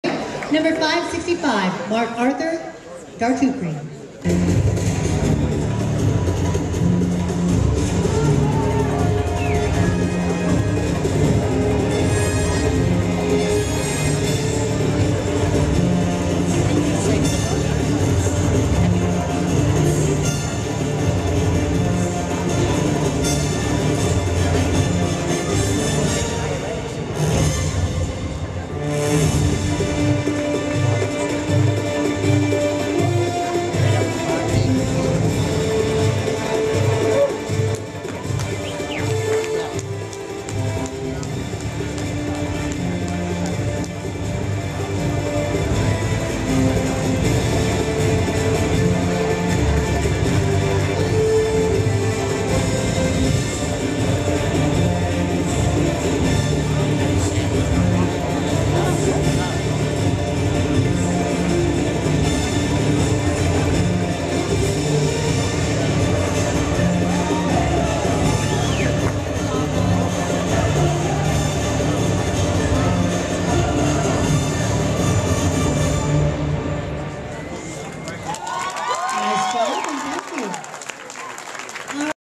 Marc Arthur Dautruchee - Best Posing Routine 2015
Team Universe 2015 Marc Arthur Dautruchee, Heavyweight - Posing Routine.